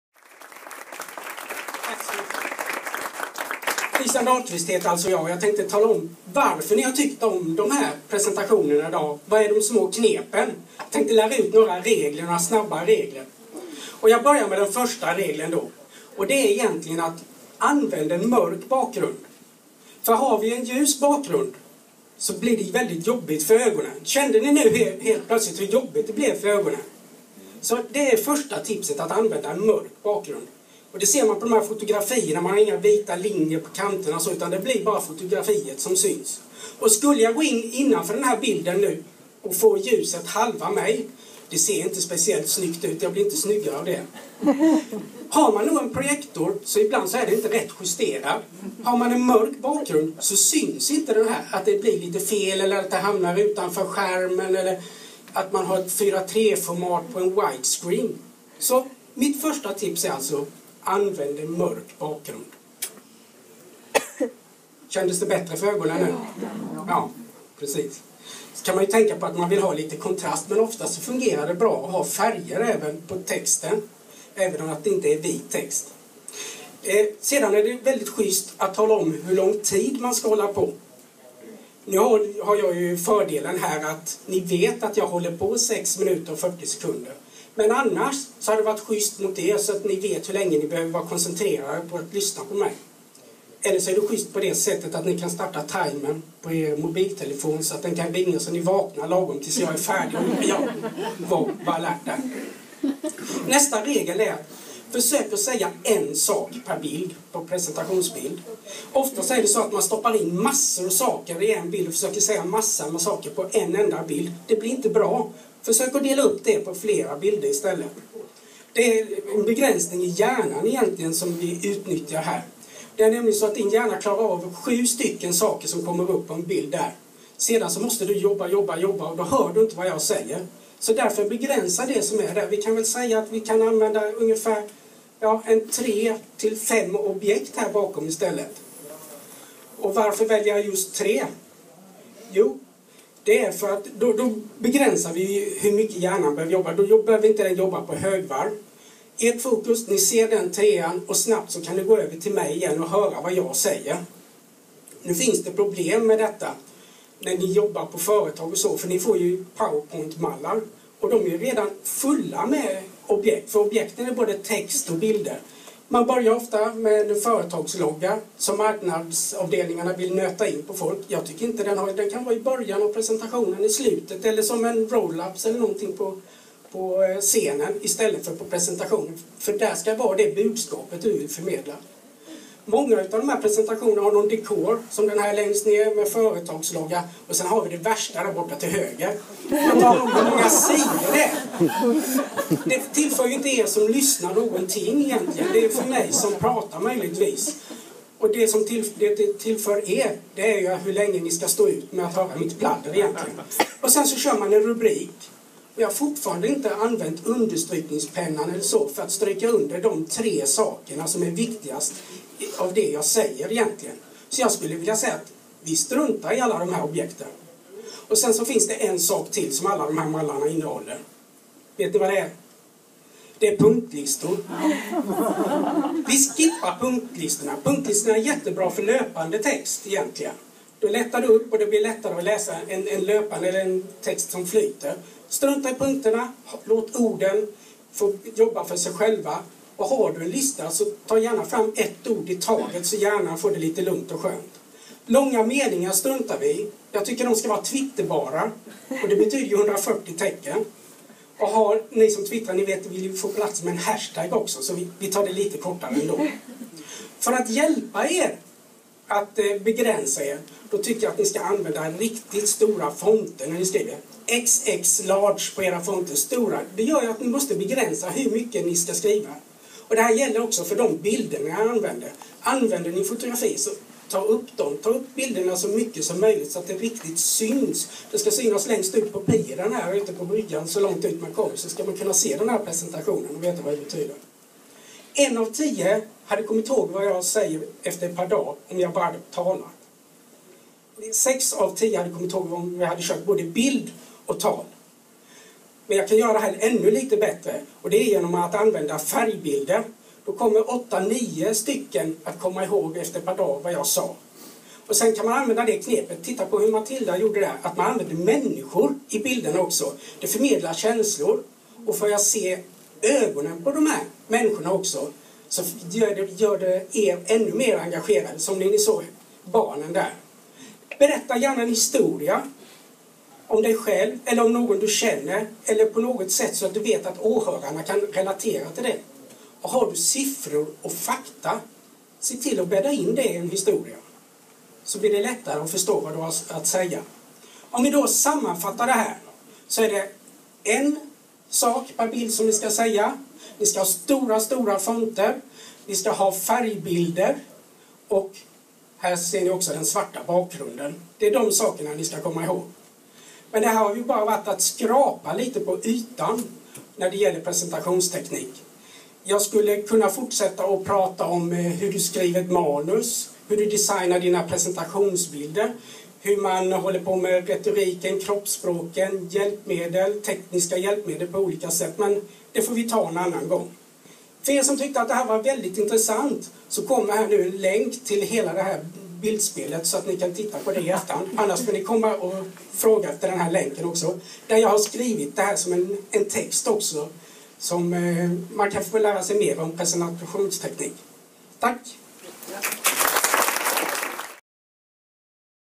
Presentationsteknik på 6 minuter
På 6 minuter får du de bästa tipsen för att göra din PowerPoint lite bättre. Jag som delar av mig av tipsen heter Christian Dahlqvist, och brinner för det här med att hur presentationer kan göras bättre.

Det hela är inspelat på den första Pecha Kucha Night i Falkenberg som hölls den 11 juni 2014 på Strandbaden i samma stad. Det är filmat med en iPad av Michael Sillion.

Känner du att ni på ert företag vill ha fler tips kan du höra av dig till mig. Du hittar mig på